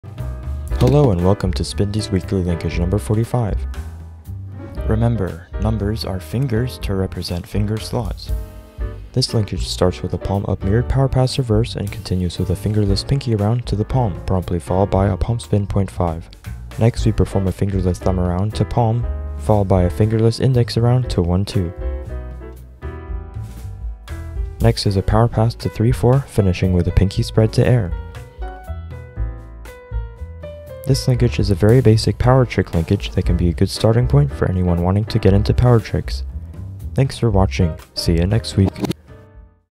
Spindy's Pen Spinning Weekly Linkage 45
Spindy’s Pen Spinning Weekly Linkage #45 is a basic power trick linkage with some east power tricks and simple linking methods.

Every week a pen-spinning link is posted on a Discord server called Spindy’s. This series is designed to break down and explain these linkages to help spinners learn notation and links.